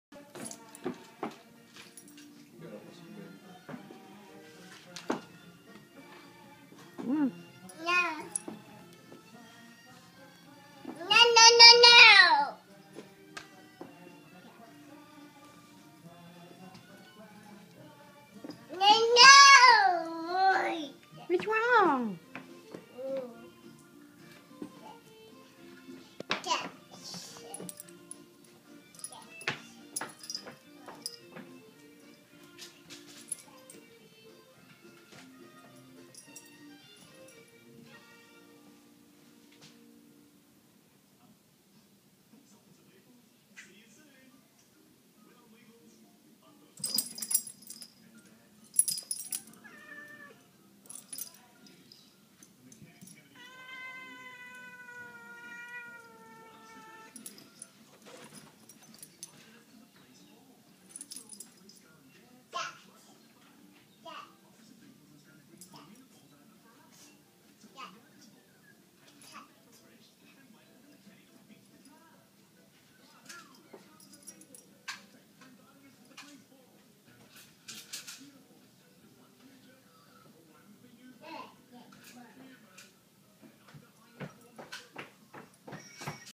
Crazy cats
Darby playing in the curtains and tackling Zazzles